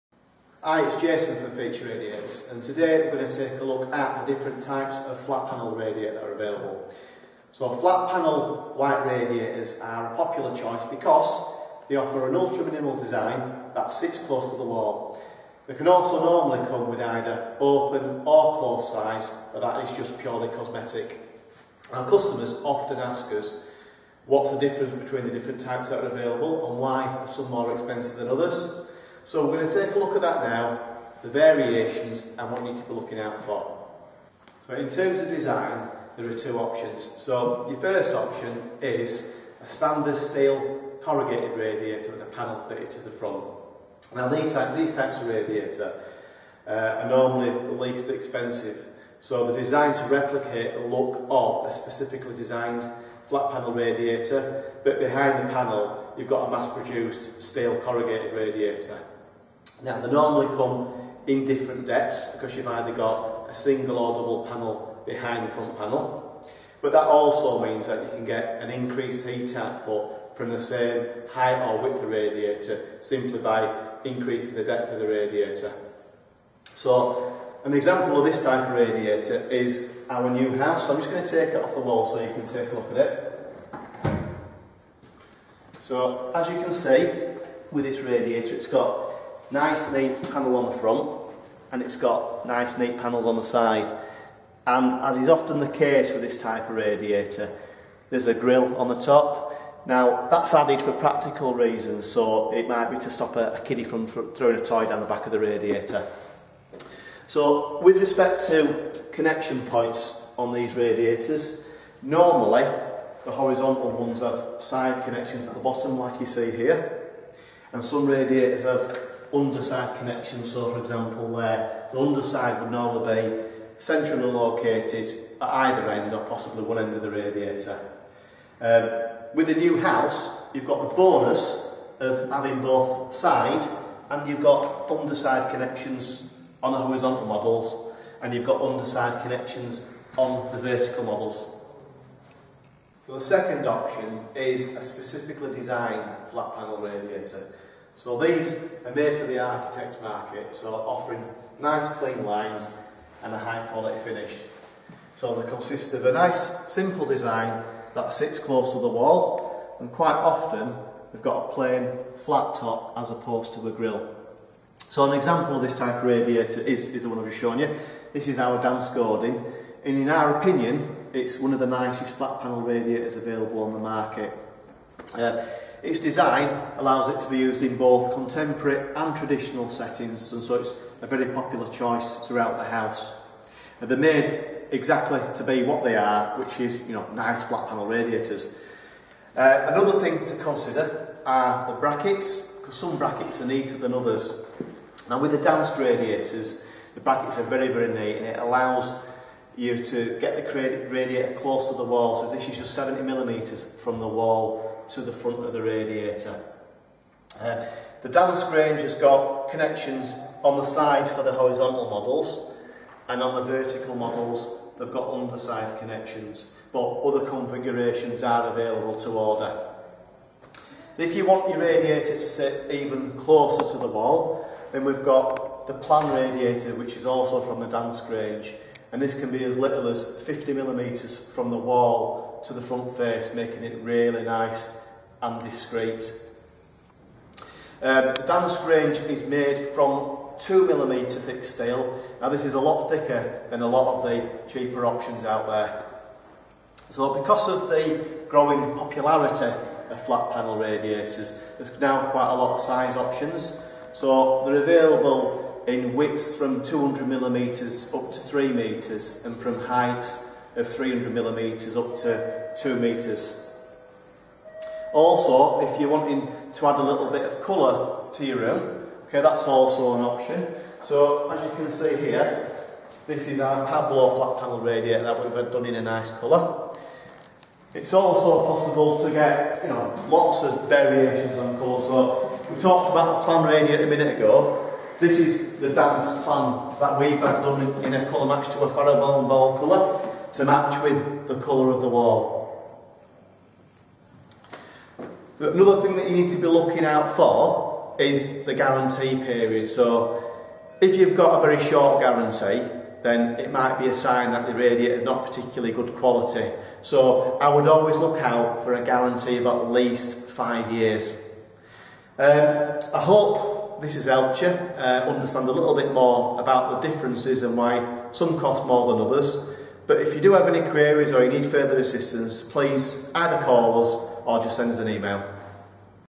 How to choose flat panel radiators
There are a wealth of minimalistic, flat panel radiators on the market, available at a wide range of prices, but how can you tell which offer good value for money?  This video from our expert, Jason, provides a clear guide on what to look for when buying a flat panel central heating radiator.

So how do flat panel radiators vary?  And what differences should you be looking out for?